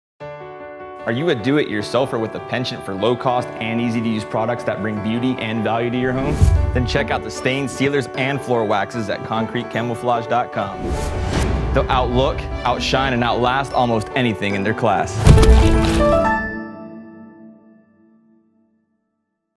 Concrete Camouflage Products Ad
Advertising high quality wood and concrete finish products that are affordable and durable.

Production Company:
Du More Films
For all your film and video needs!

Editor: Ducon Williams

Special Thanks to:
Jeremy & Shannen White
for the use of their beautiful home!